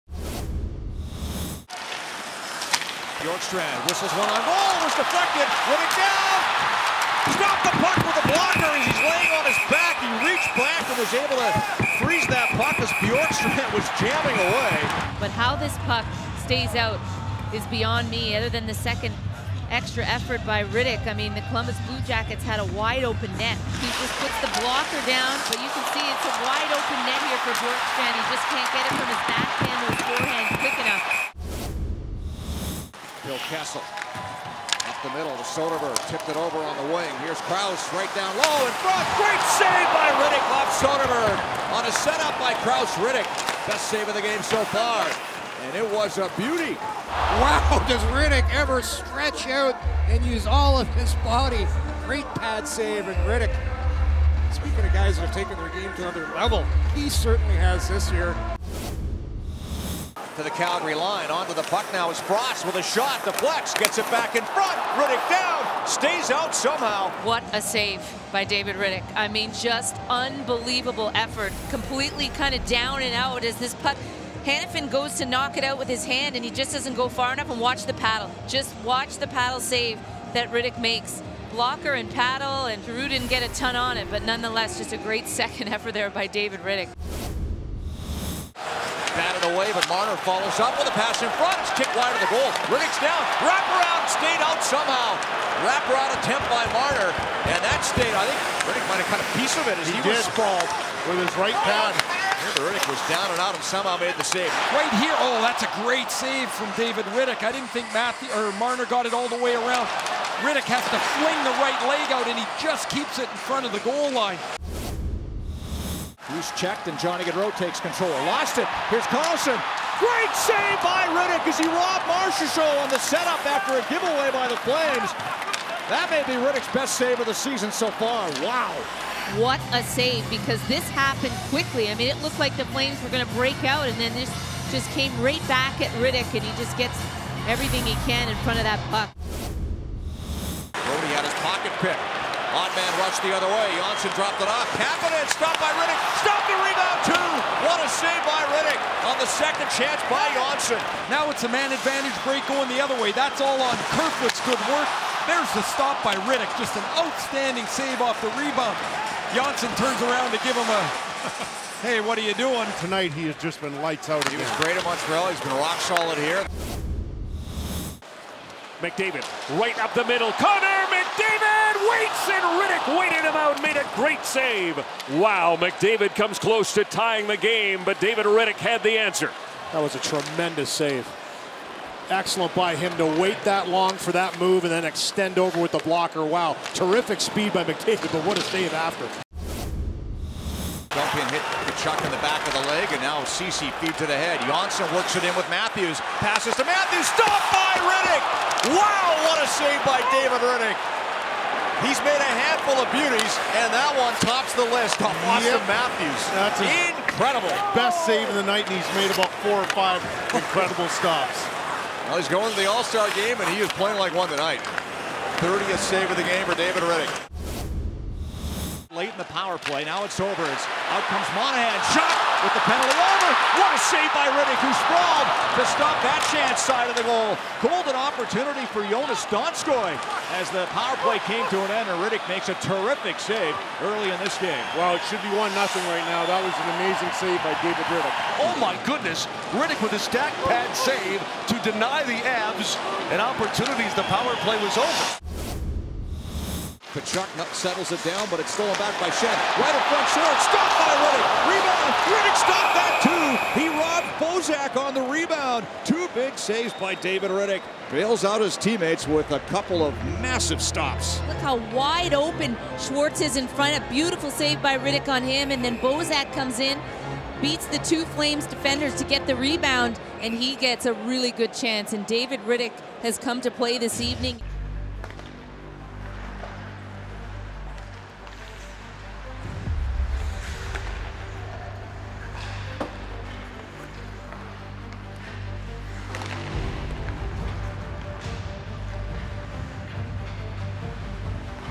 Top 10 David Rittich Saves from 2019-20 | NHL
Counting down the top ten highlight-reel saves in 2019-20 from Calgary Flames goaltender David Rittich

Feeling social?